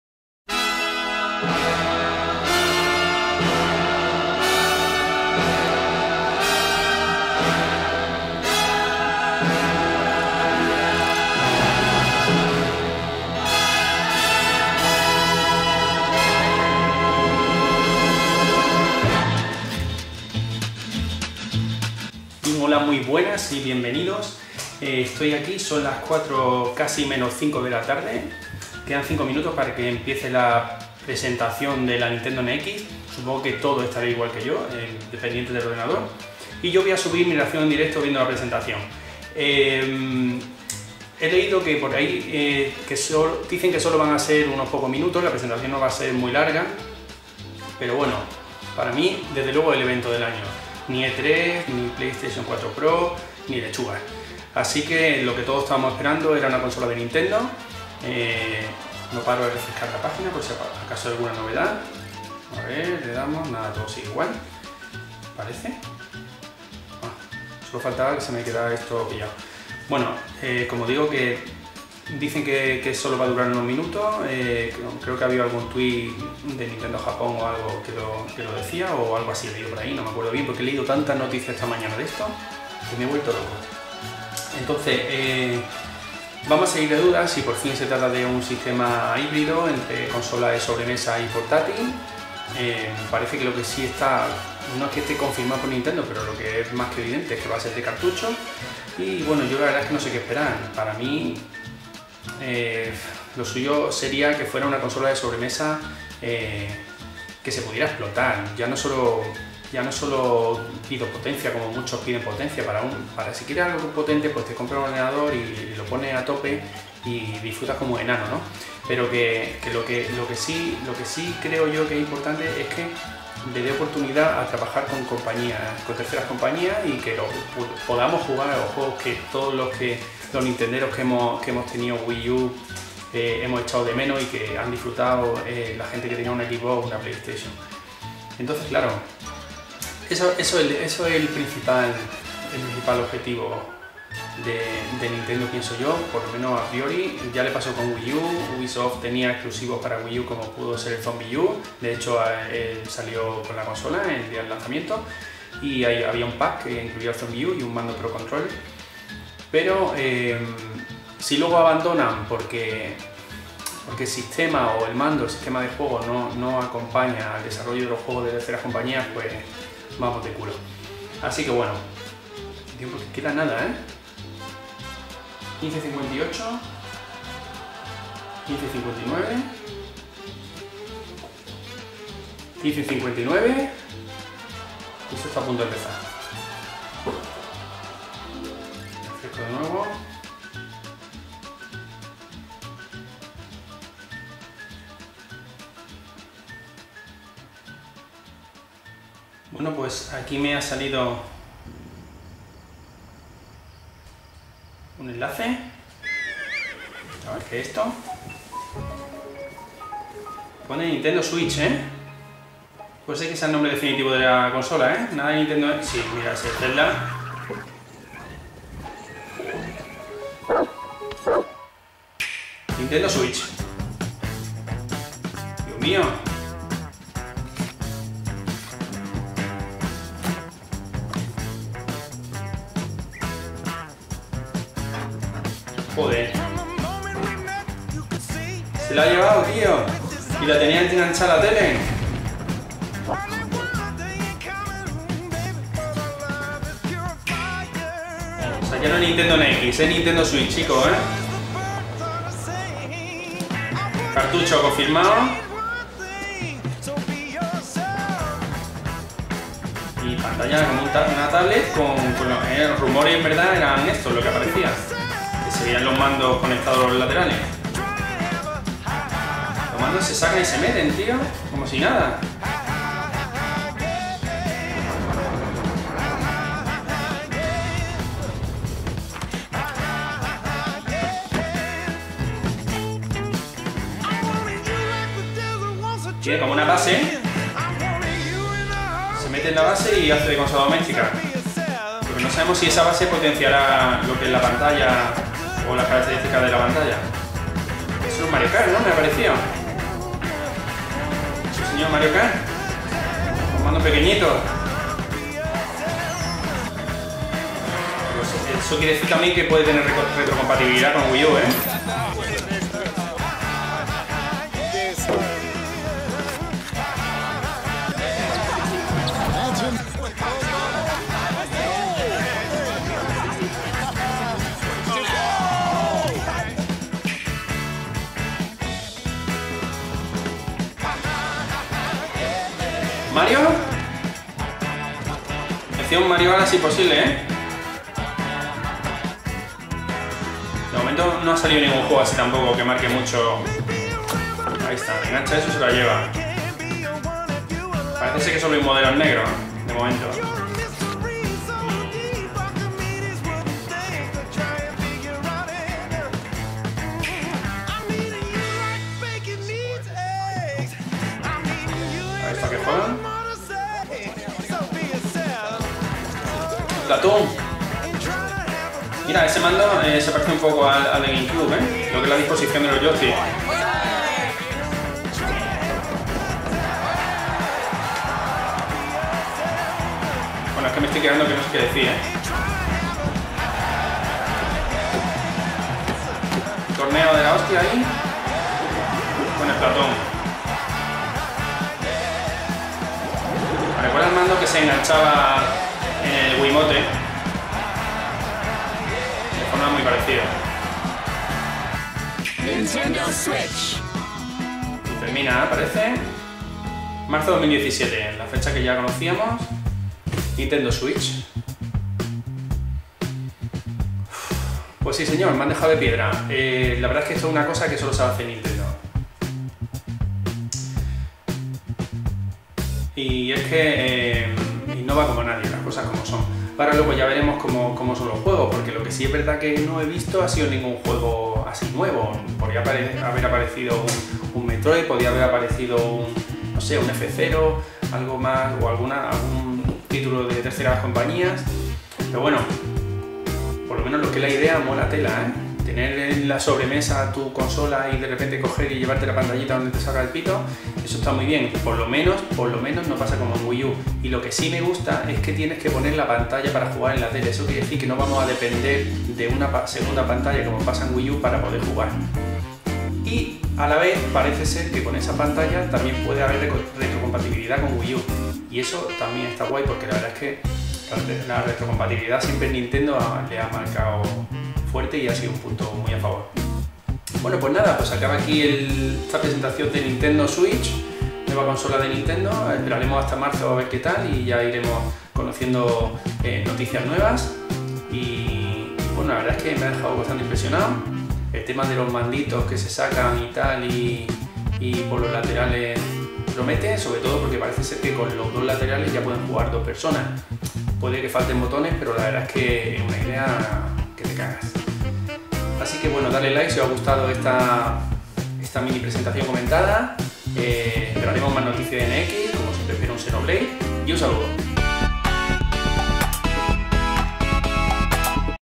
NX ES NINTENDO SWITCH !! PRESENTACIÓN - REACCION
Reacción en directo de la presentación de Nintendo NX, descubrimos la Nintendo Switch! Solo Nintendo sabe hacer estas maravillas, solo Nintendo sabe innovar!!

búscame en twitter @jemanmaster